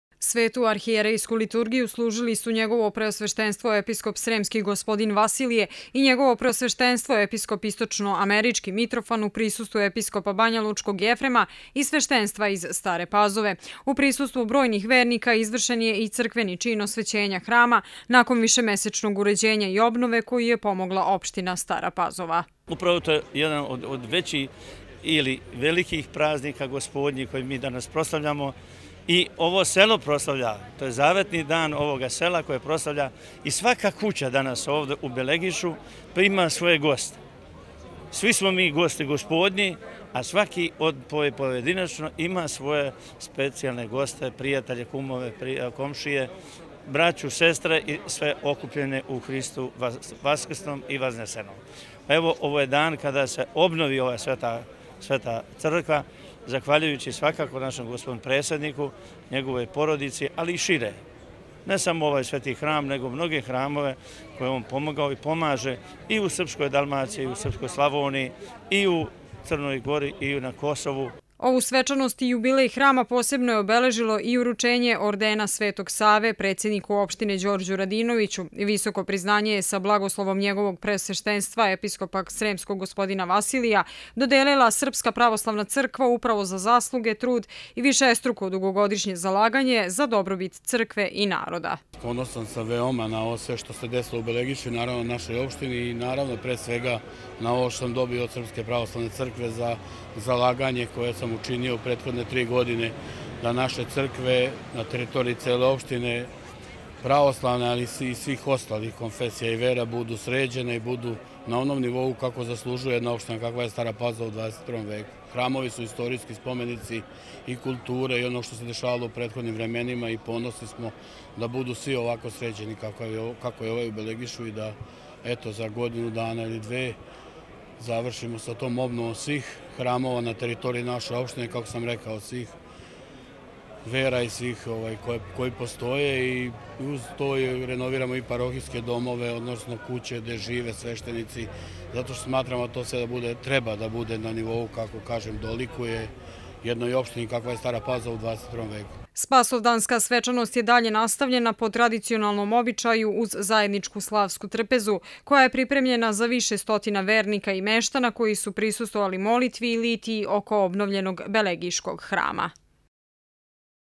Obnovljen hram u Belegišu
Litijom glavnom ulicom Belegiša a zatim i Svetom Arhijerejskom Liturgijom u hramu Prenosa moštiju Svetog oca Nikolaja  danas je počelo obeležavanje mesne slave Spasovdana.